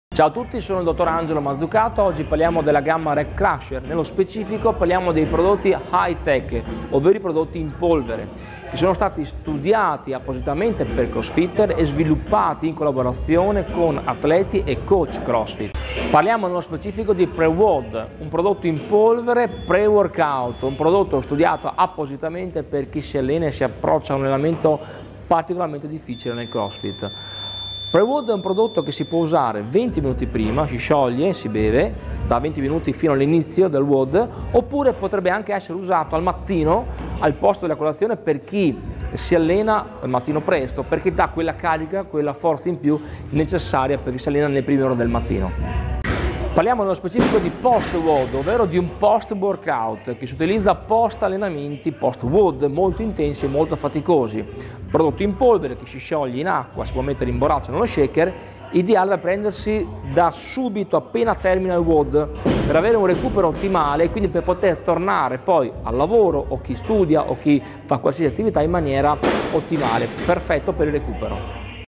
Rep Crusher - Pre Wod e Post Wod
Il Dott. Angelo Mazzucato illustra Pre WOD e Post WOD, l'integrazione completa studiata da ProAction per tutti gli atleti CrossFit®